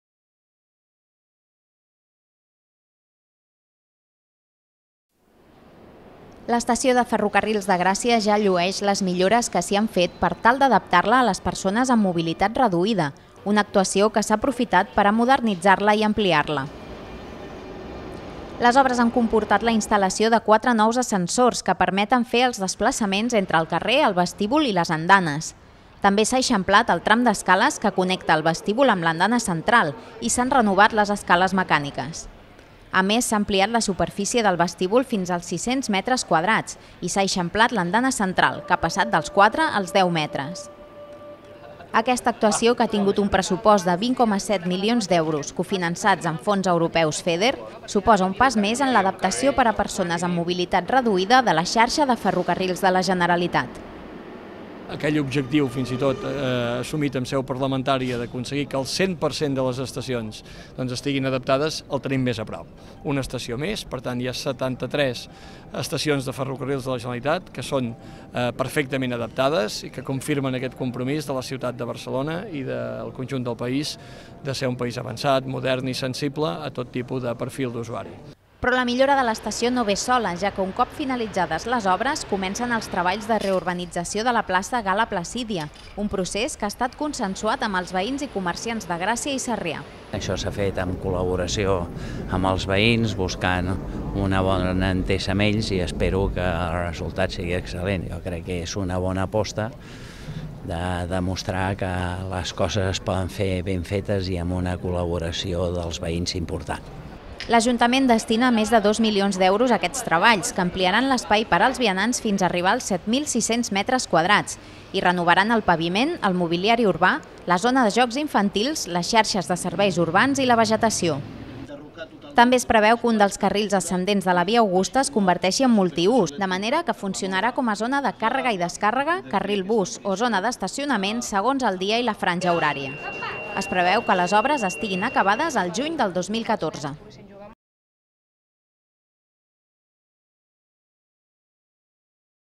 La fi de les obres a l'estació de Gràcia dóna pas a la reurbanització de Gal·la Placídia
Amb aquests treballs s'ha adaptat l'estació a les persones amb mobilitat reduïda, També s'ha ampliat i modernitzat l'espai.